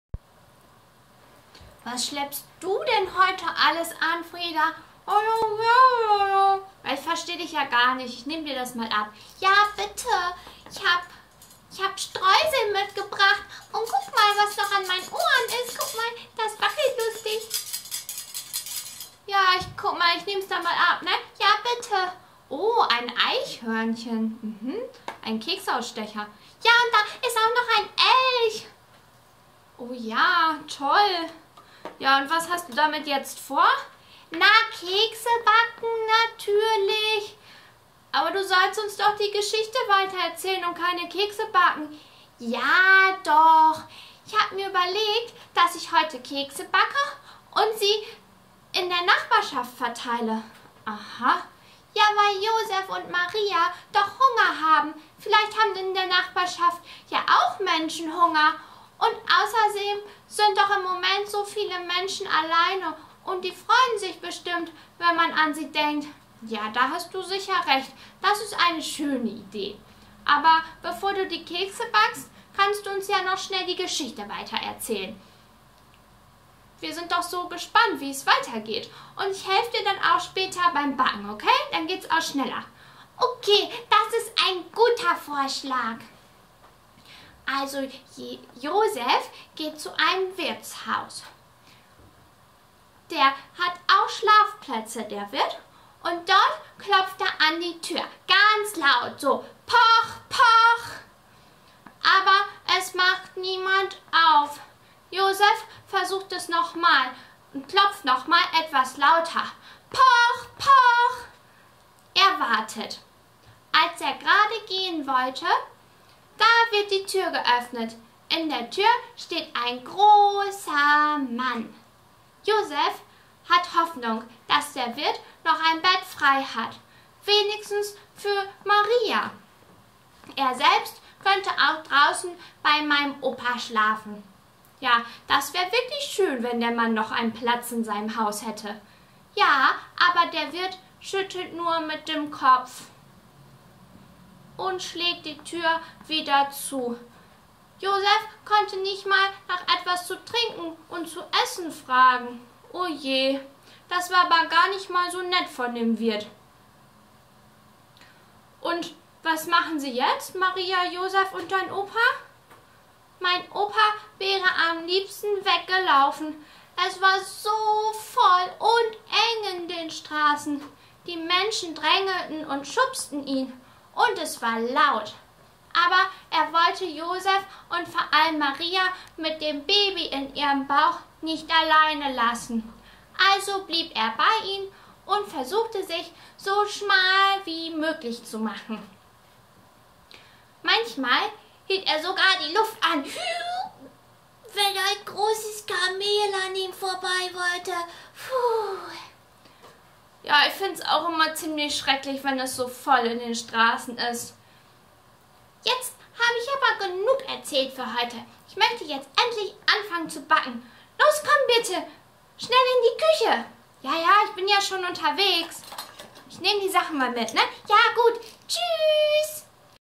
Adventskalender, 12. Dezember
"Frieda erzählt..." - Ein Adventskalender nach der Weihnachtesgeschichte (Lk 2,1-20).

Text: Birte Kochsiek (Diakonin im Berufsanereknnungsjahr),  Cuxhaven 2020